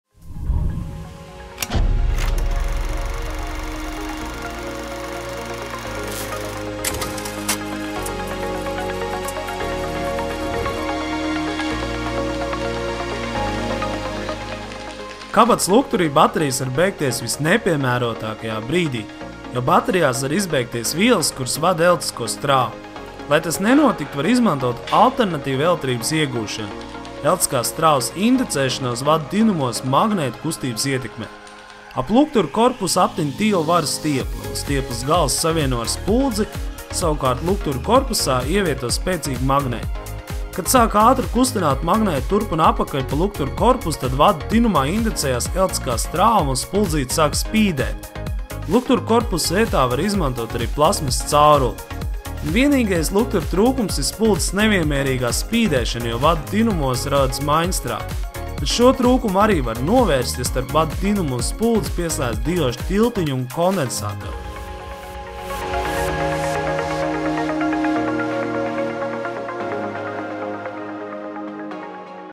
Lukturis bez baterijām
Skaties, kā var iegūt lukturīti, neizmantojot baterijas!